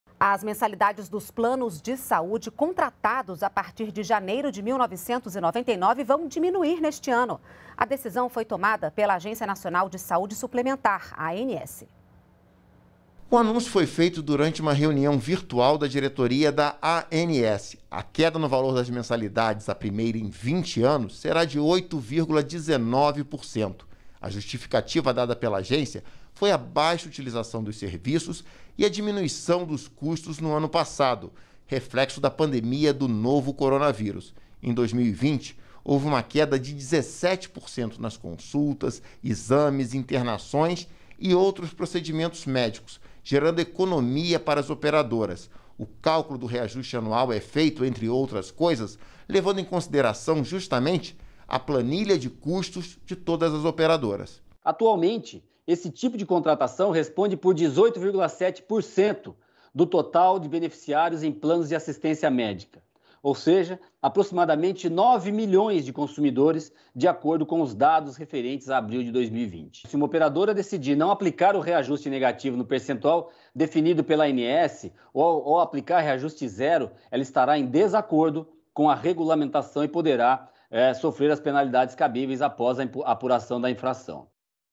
Planos de saúde individuais vão ficar mais baratos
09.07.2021 - Os usuários de planos de saúde individuais contratados a partir de janeiro de 1999 terão um reajuste negativo esse ano. Isso significa que, nos próximos meses, as mensalidades ficarão mais baratas. A decisão foi da Agência Nacional de Saúde Suplementar (ANS).